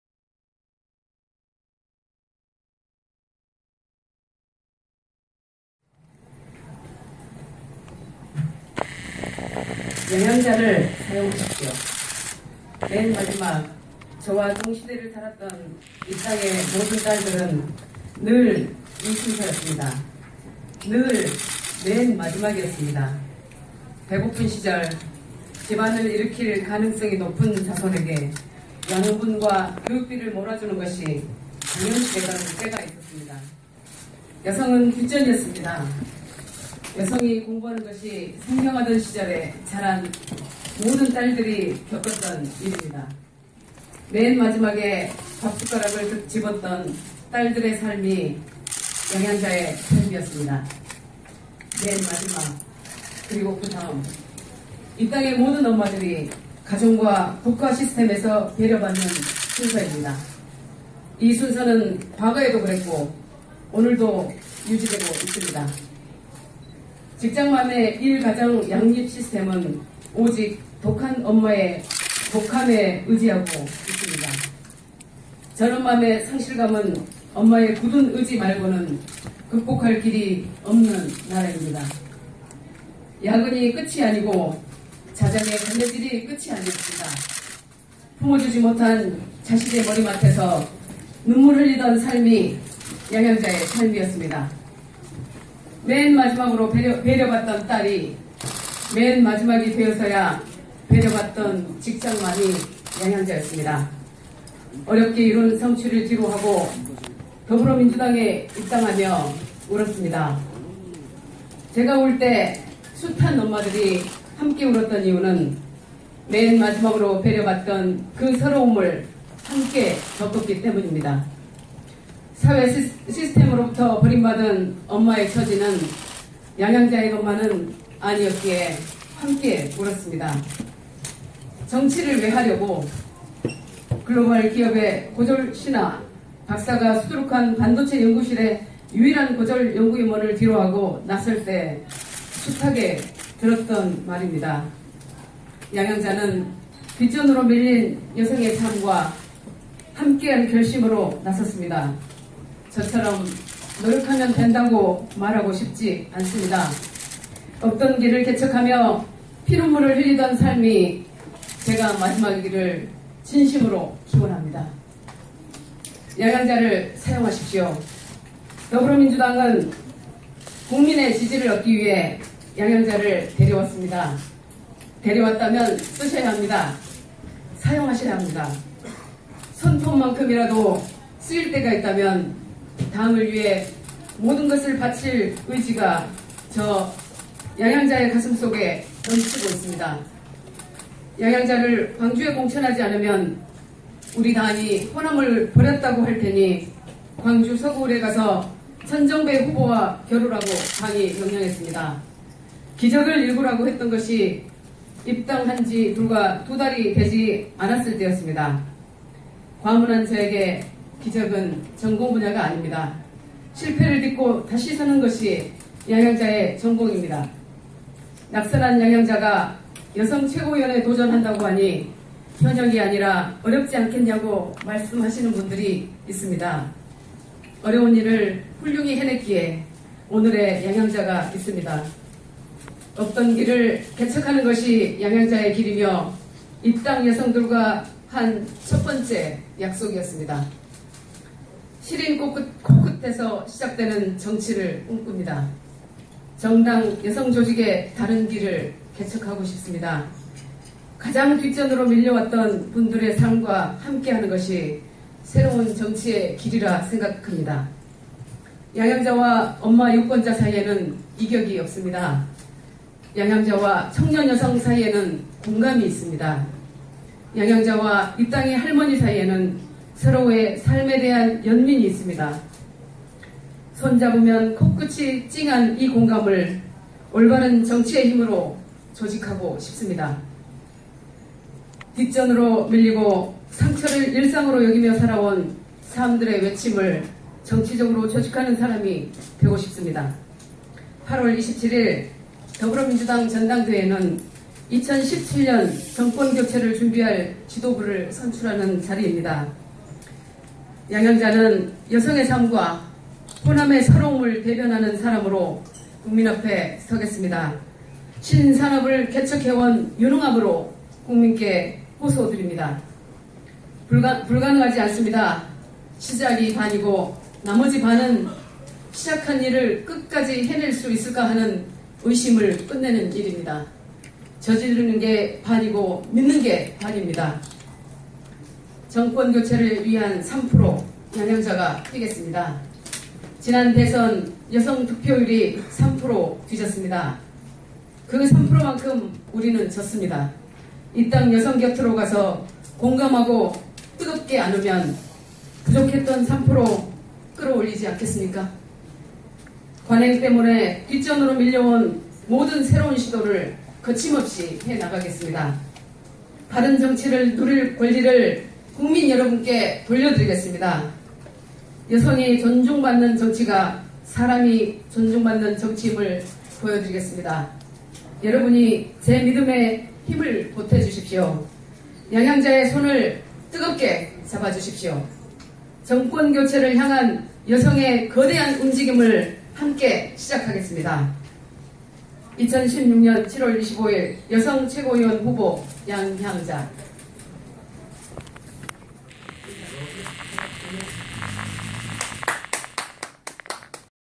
2016 07 25양향자 최고위원 출마 기자회견
더불어민주당 당 대표보다 더 치열한 최고위원 선거에 여성위원장 후보로 출마선언하는 양향자 전 상무 양향자 유은햬 양자구도로 대결양상